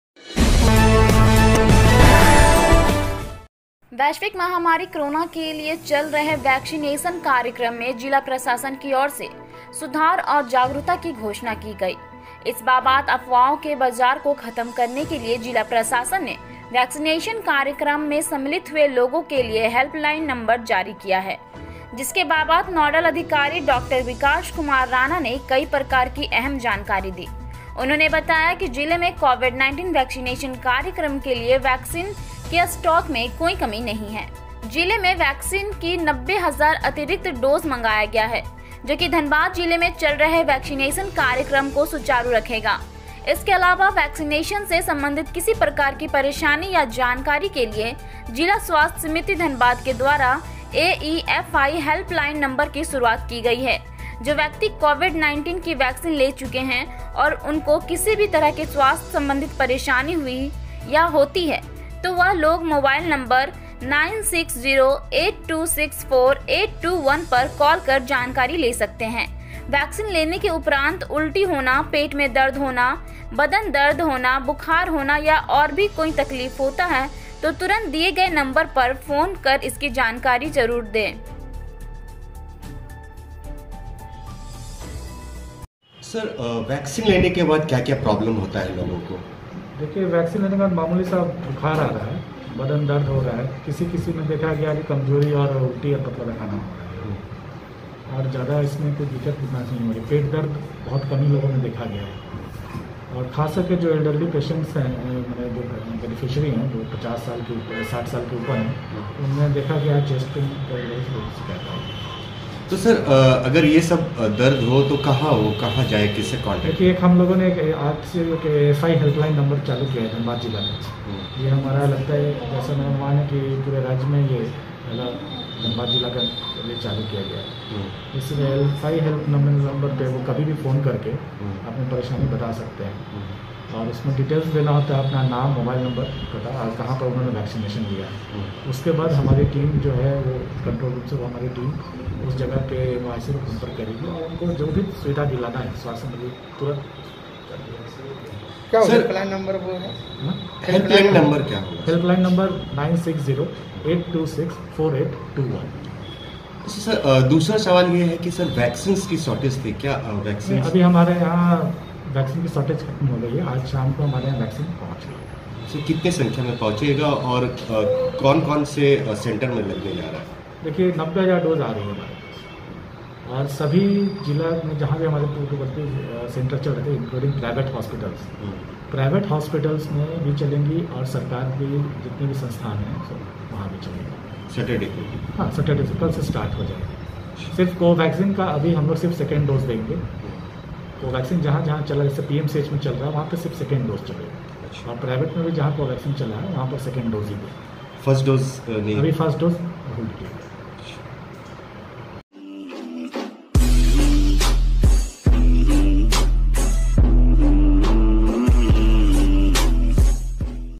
वैक्सीनेशन कार्यक्रम में सम्मिलित हुए लोगों के लिए हेल्पलाइन नंबर जारी
वैक्सीन लेने के उपरांत उल्टी होना,पेट दर्द होना,बदन दर्द होना,बुखार होना या और भी कोई तकलीफ हो तो तुरंत दिए गए नंबर पर फ़ोन कर इसकी जानकारी जरूर दें।